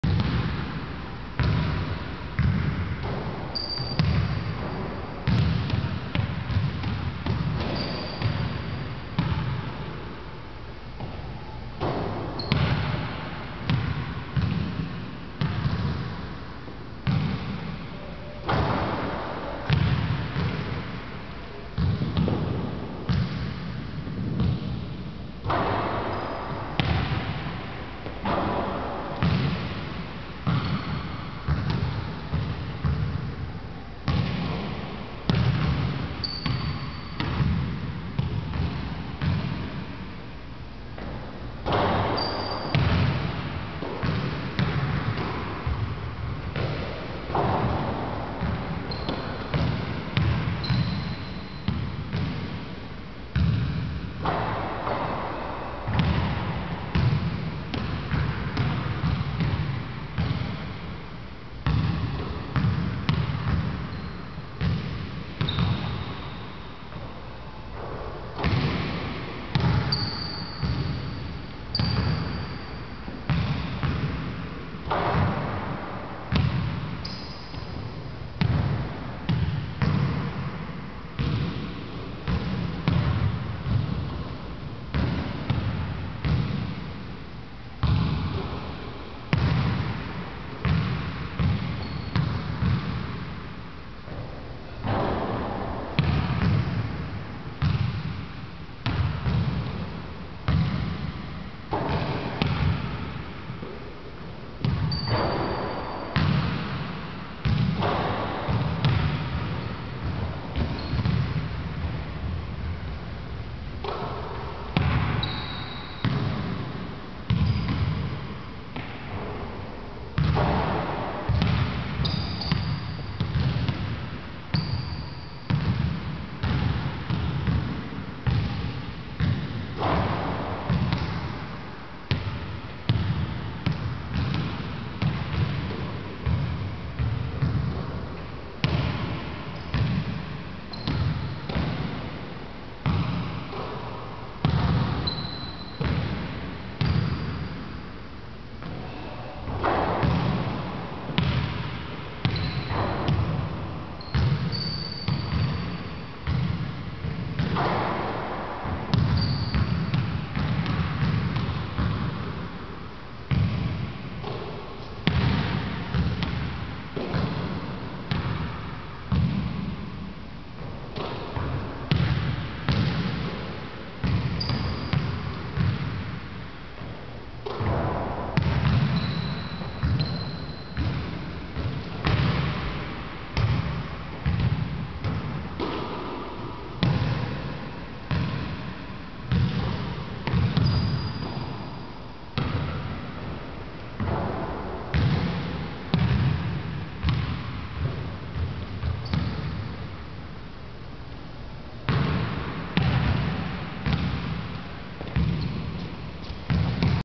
NBA Mid-range practice on one leg!
Footage of me practicing my mid-range shot.
Don't be fooled by my walking boot. I only strained my Achilles Tendon, but the doctor insisted that I wear a walking boot.